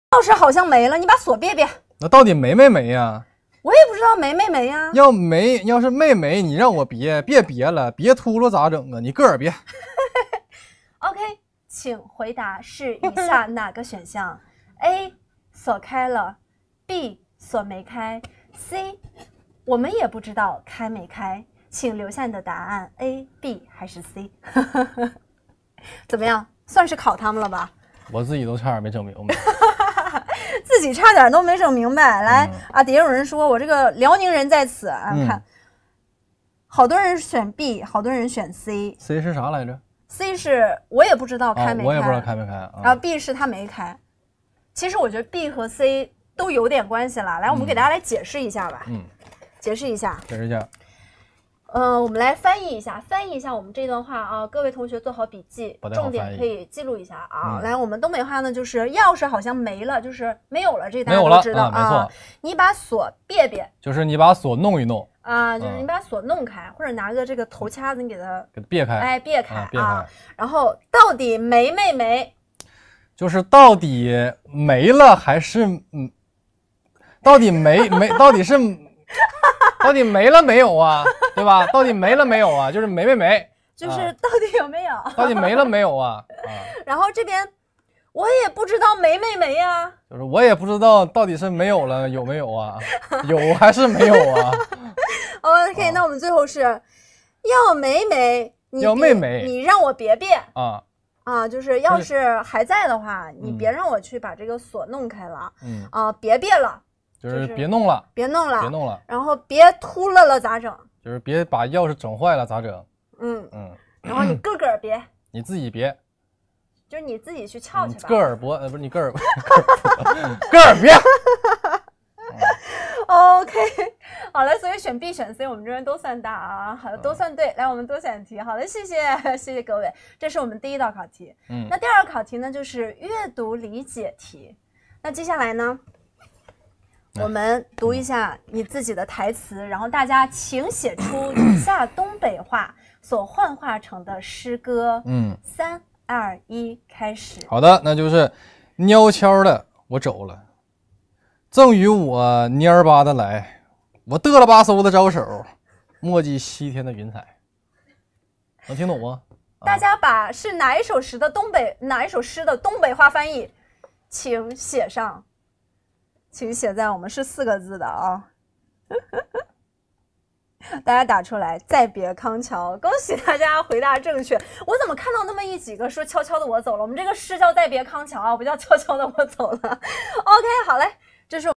全辽宁最有种的男人为东北话代言#李宏毅#3CE商务京东直播
百变玩妆 毅起出彩#京东直播
很高兴成为3CE品牌体验官，快和我一起探秘@3CE_STYLENANDA 38节日限定新品，妆点春日浪漫时刻。